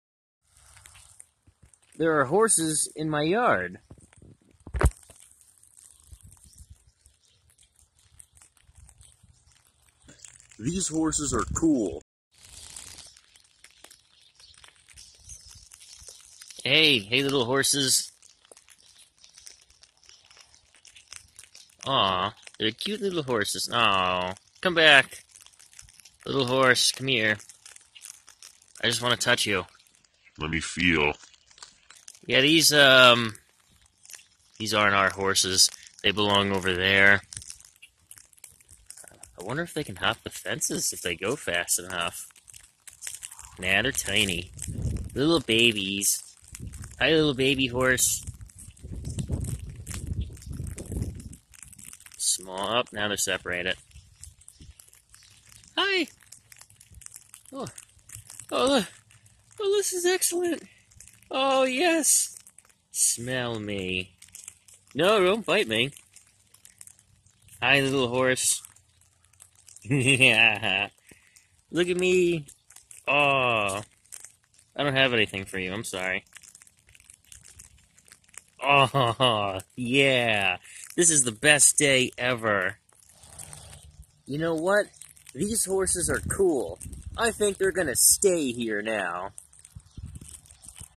Mini Horses Wander into my Yard
Two mini horses from the ranch next door came to visit The Freedom Zone. They are small, wholesome, and cute.